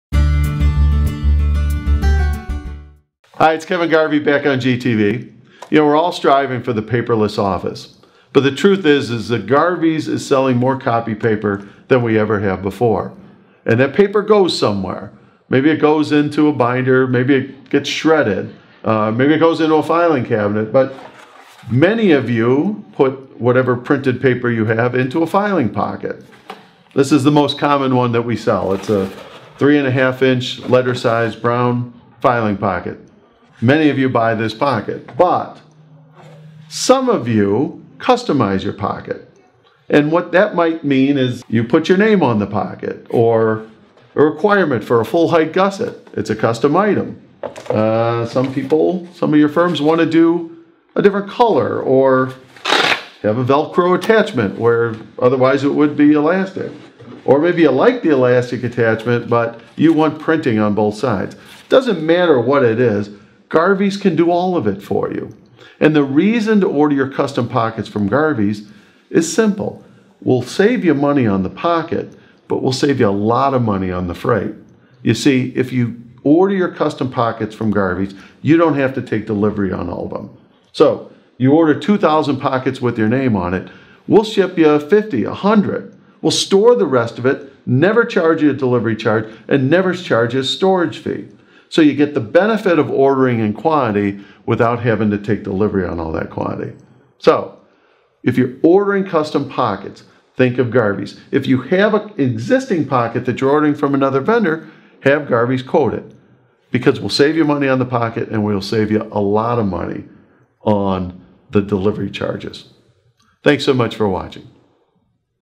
Custom File Pockets
Need a custom file pocket your business? The Nice Guys can help! Garvey’s Office Products provides solutions for all types of branded material, including filing pockets.